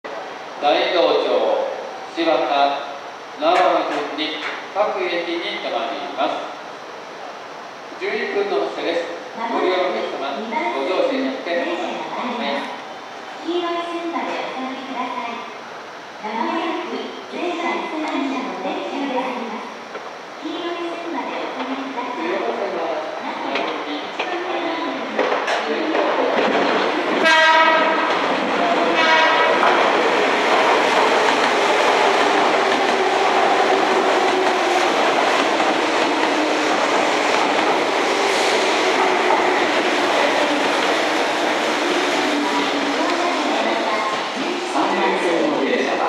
【3分遅れから生じた奇跡？】全車一般車VS一部特別車 特急同士の並走！
説明  名鉄5300系と名鉄1000・1200系が並んで入線。
豊橋から伊奈間にて強風の影響によって本線から来る車両は3分の遅れが生じました。それにより普段の時間差とは少々異なるため、このような事態が起きました。